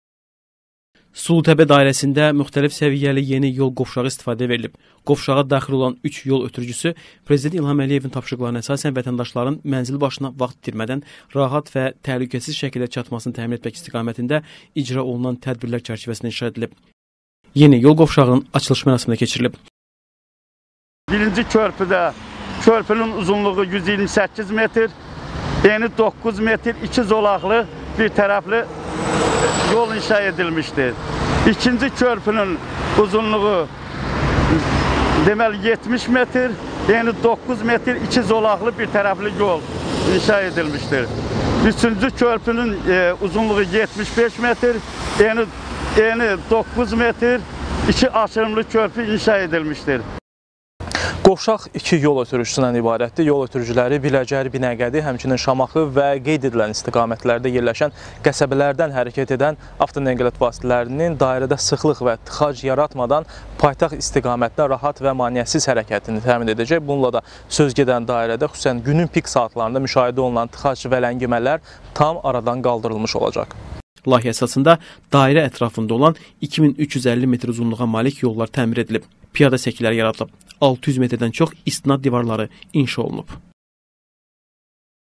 Yol qovşağı istifadəyə verilib
Sulutəpə dairəsindəki qovşaq 2 yol ötürücüsündən ibarətdir